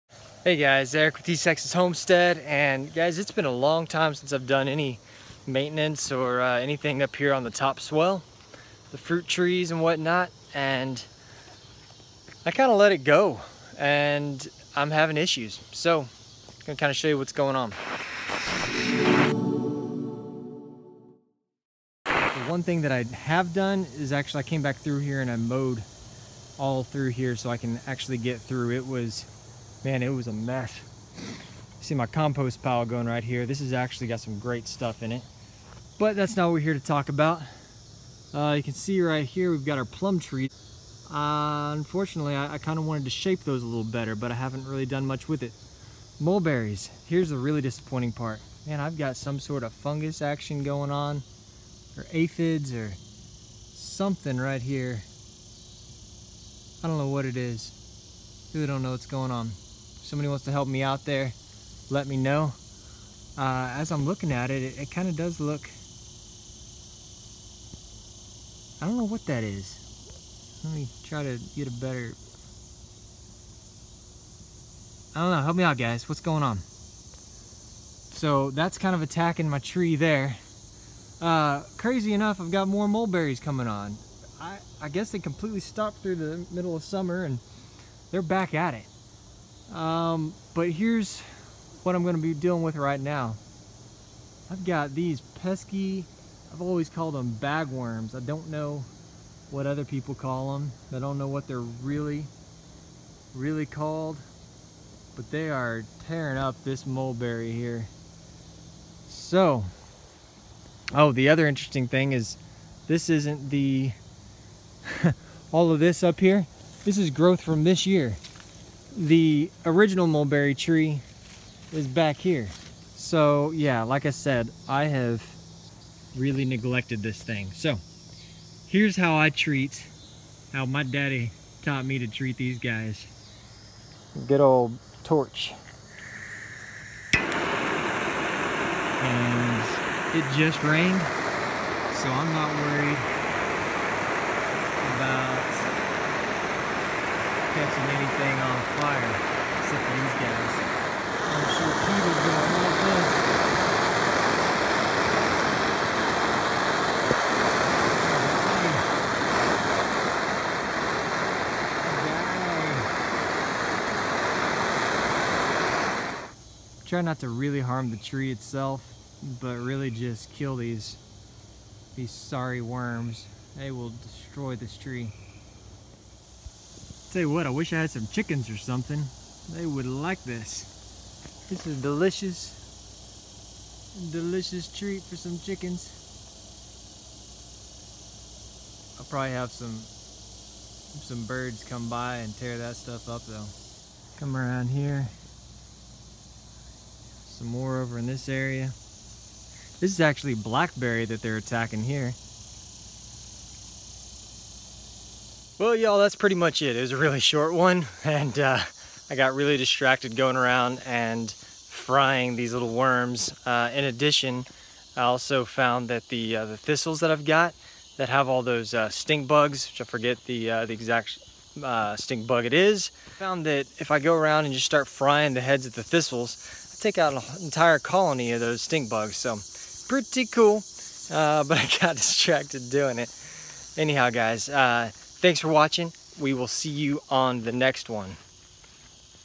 🔥🐛 Killing Bag Worms With a Torch! 🐛🔥
This is a really quick video about killing bag worms with a torch. I'm not sure what these things are actually called. I grew up calling them bag worms but I've heard others call them web worms.

All I know is that Fire is the easiest way to destroy these little tree killing worms. What's your favorite way to kill bag worms?

This tool is how I outrank much larger channels!

Category